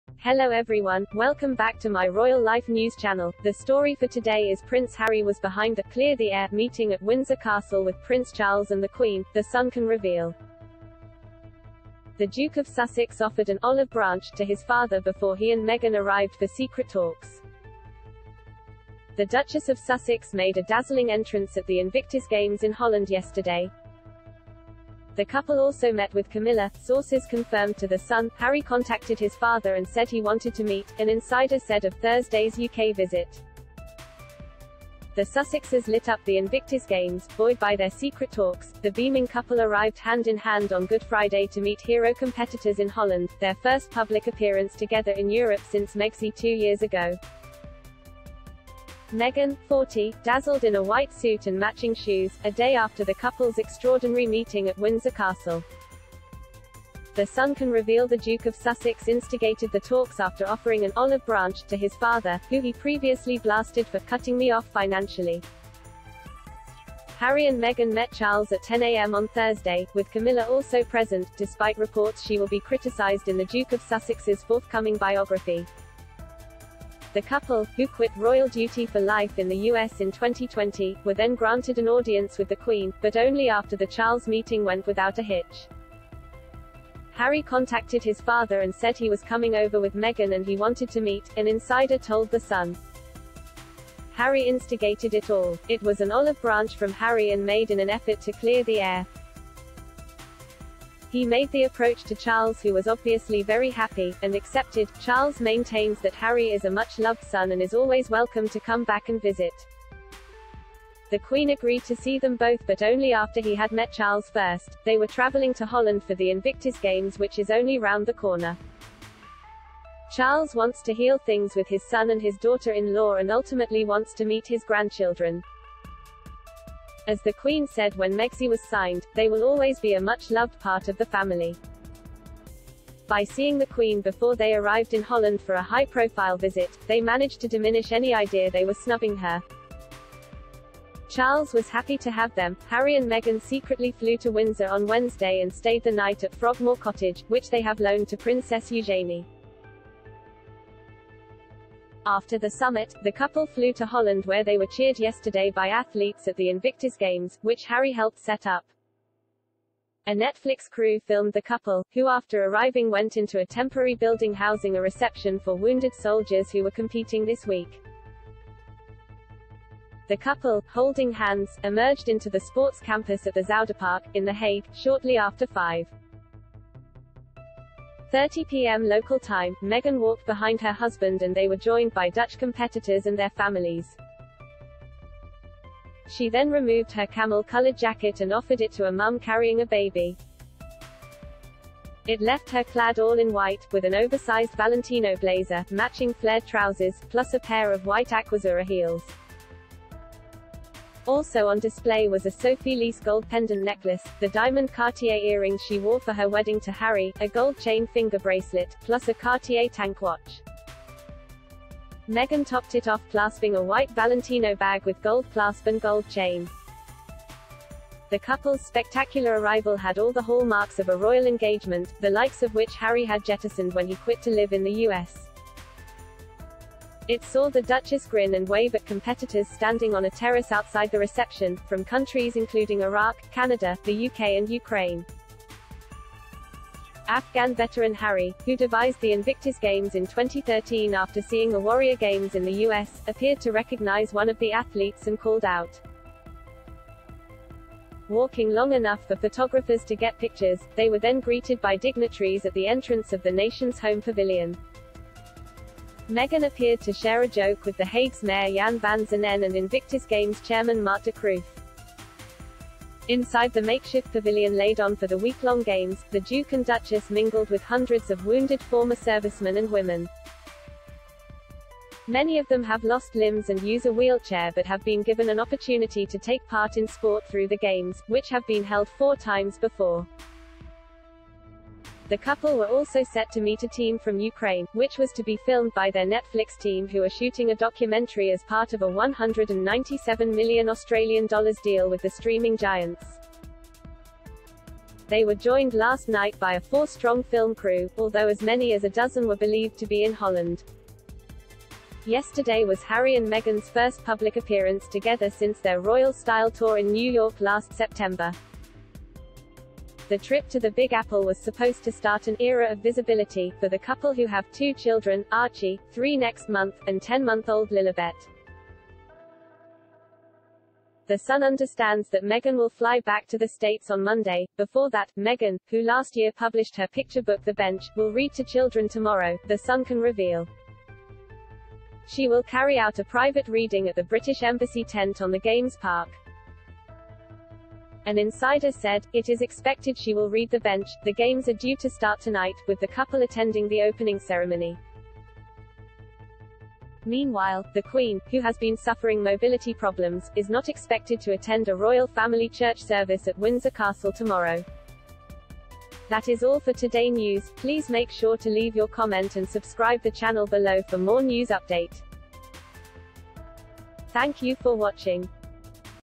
Inside Prince Harry, Meghan Markle’s secret talks with Prince Charles
New details have emerged about Harry and Meghan’s secret talks with Prince Charles – including who was behind their meeting to “clear the air”.